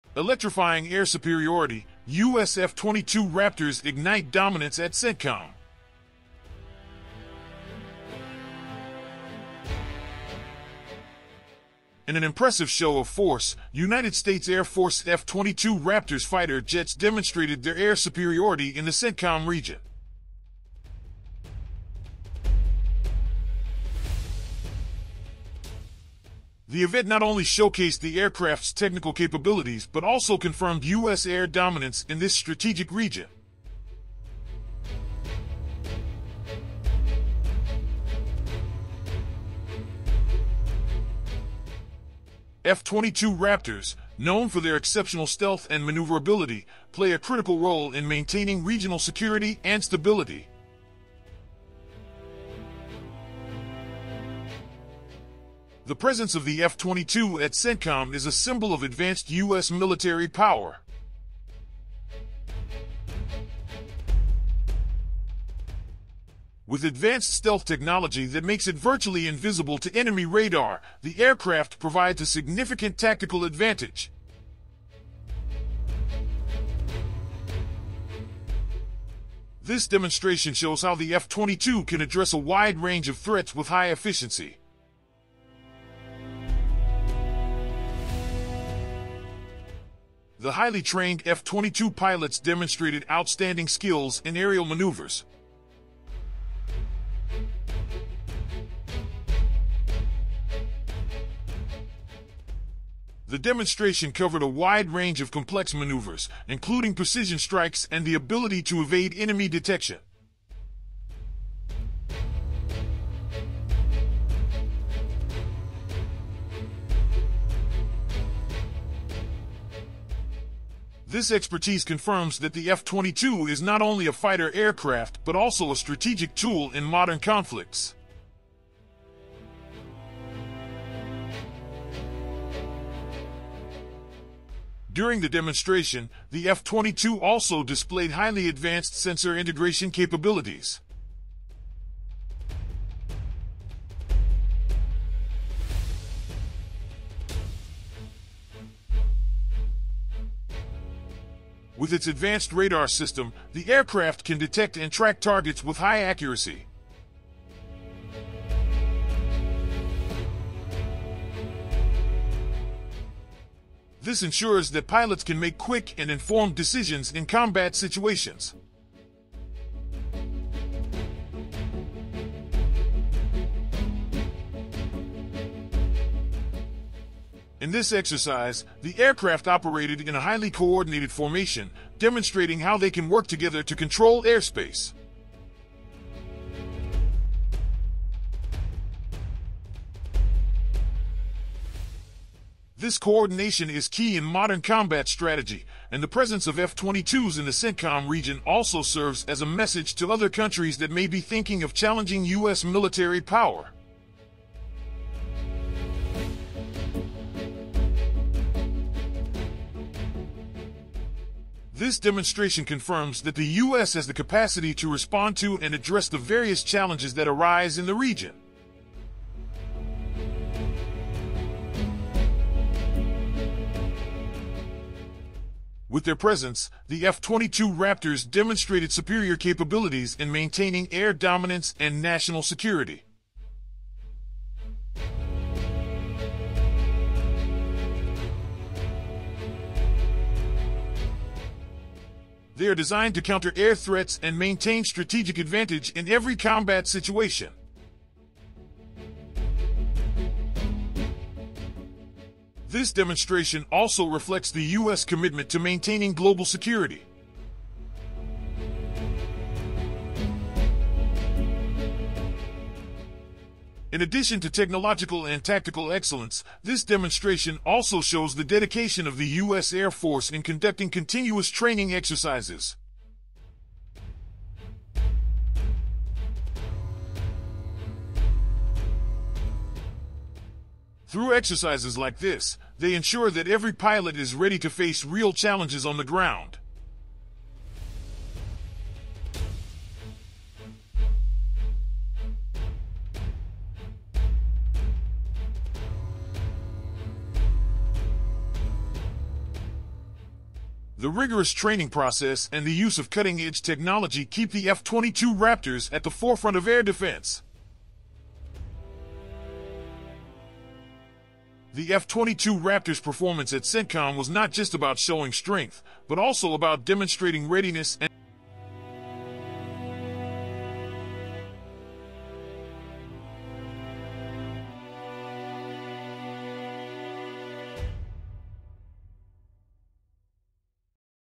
Electrifying Air Superiority US F-22 Raptors Ignite Dominance at CENTCOM
In an impressive show of force, United States Air Force F-22 Raptors fighter jets demonstrated their air superiority in the CENTCOM region. The event not only showcased the aircraft's technical capabilities but also confirmed US air dominance in this strategic region.